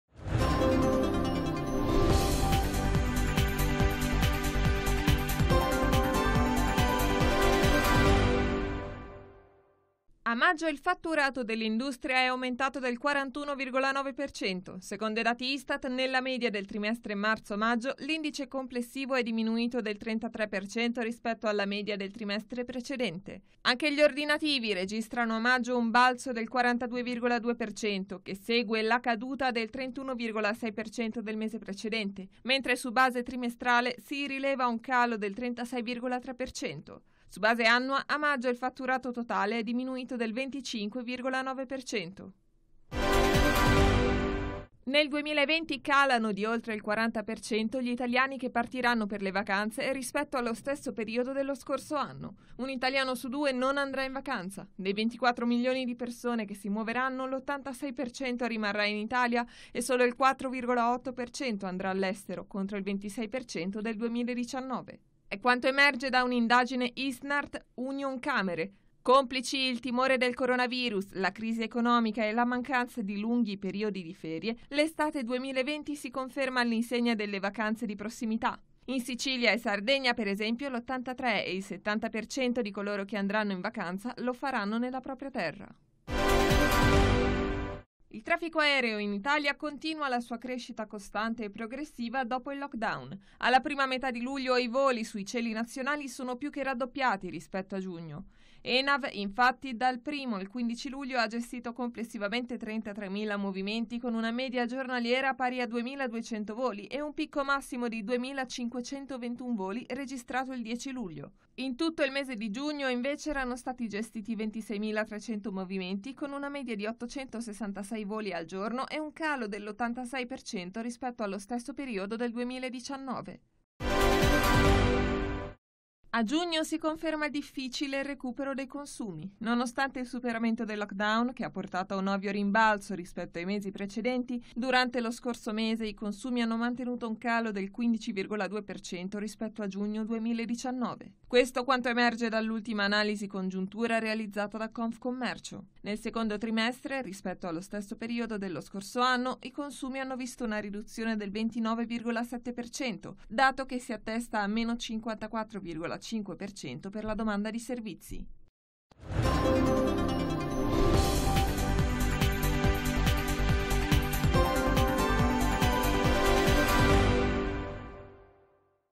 Tg Economia - 17/7/2020
In questa edizione:

- Industria, a maggio fatturato +41,9%
- Turismo, un italiano su due non andrà in vacanza
- A luglio raddoppia traffico aereo rispetto a giugno
- Confcommercio, i consumi faticano a recuperare